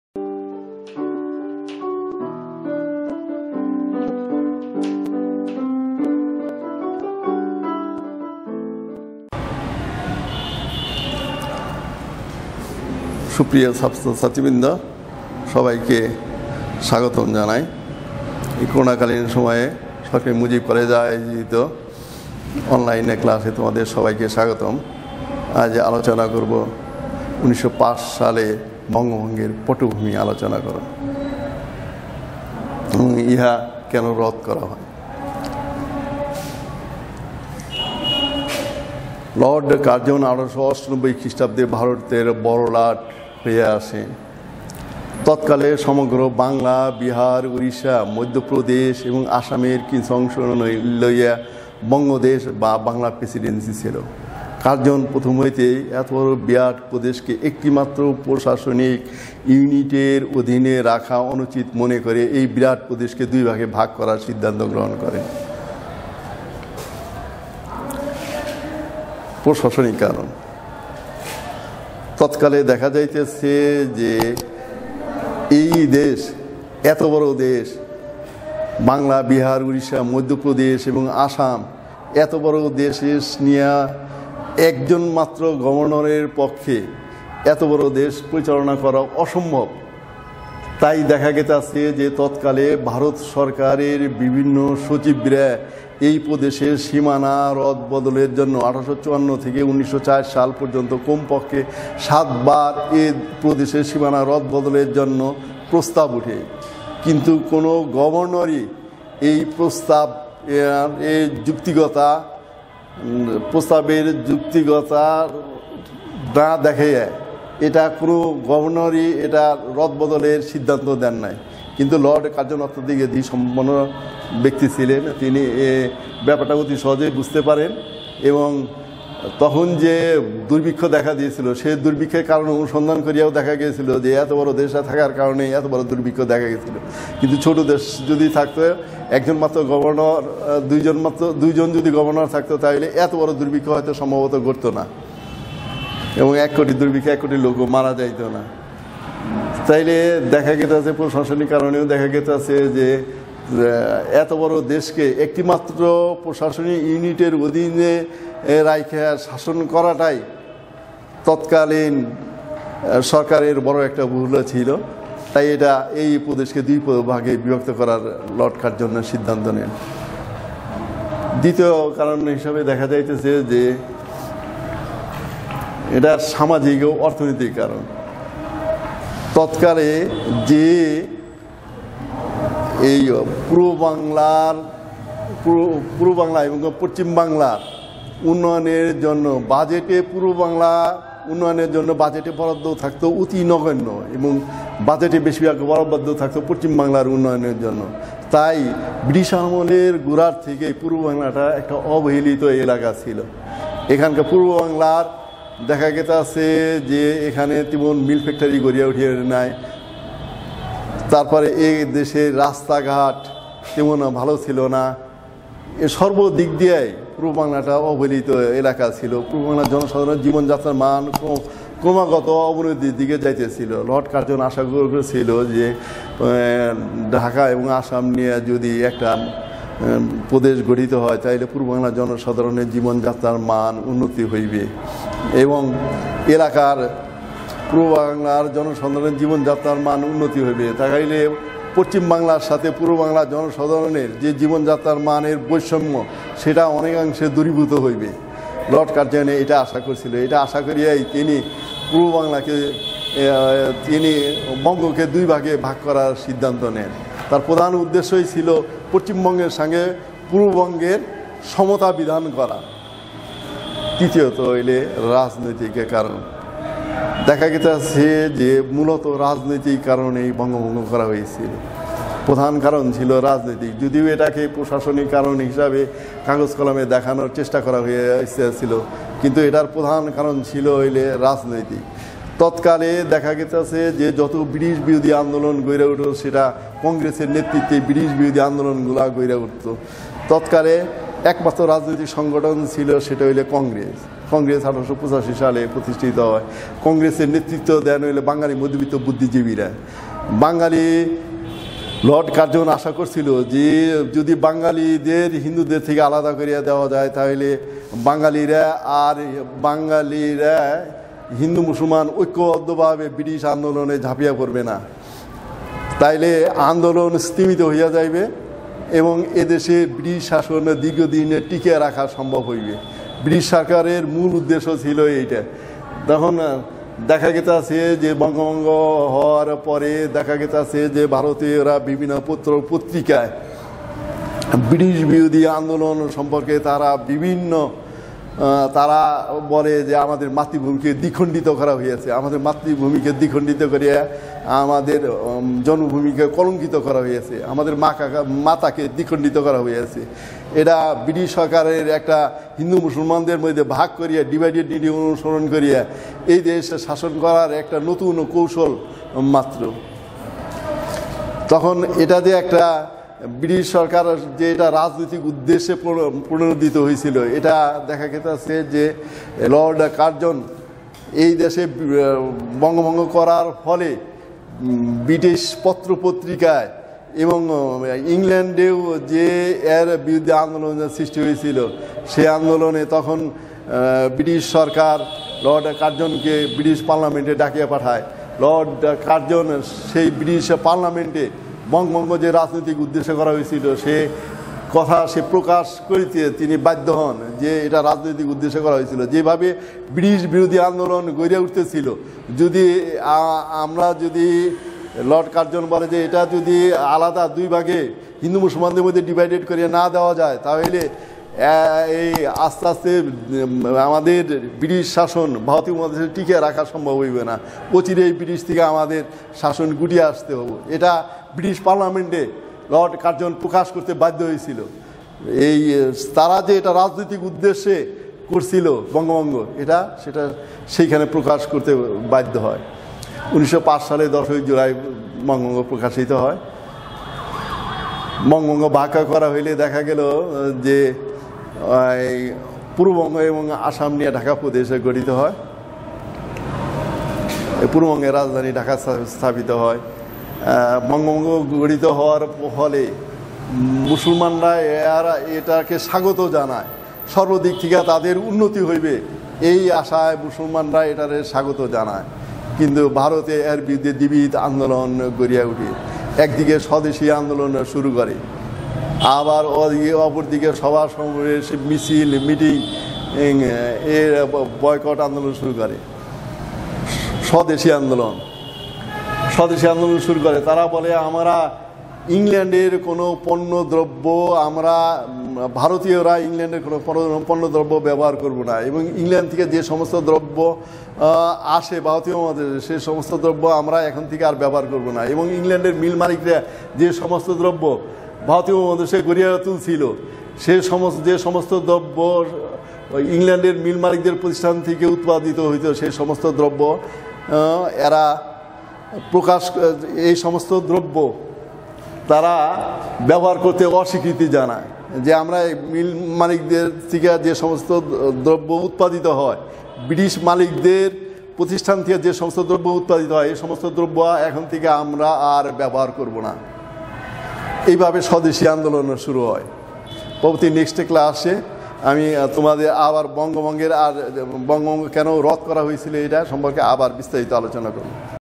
ইতিহাস ১ম পত্র। একাদশ শ্রেণি। বঙ্গভঙ্গের পটভূমি আলোচনা কর।
মোঃ আব্দুল জলিল, প্রভাষক, ইতিহাস, সরকারি মুজিব কলেজ, সখিপুর, টাঙ্গাইল।